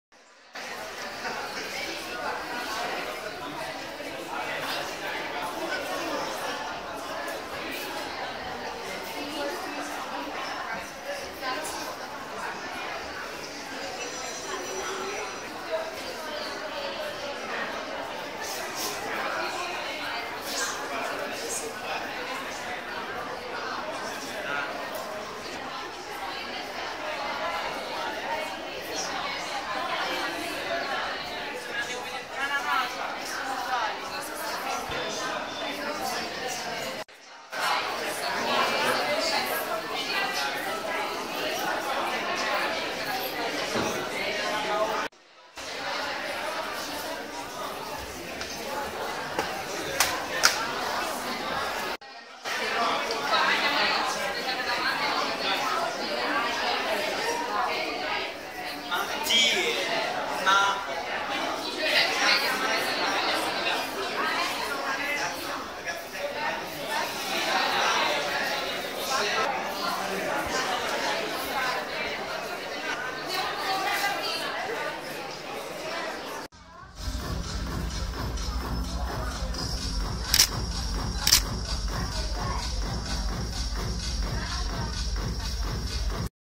Models
25 Giugno 2018

Preparazione alla sfilata NABA presso Apollo Club a Milano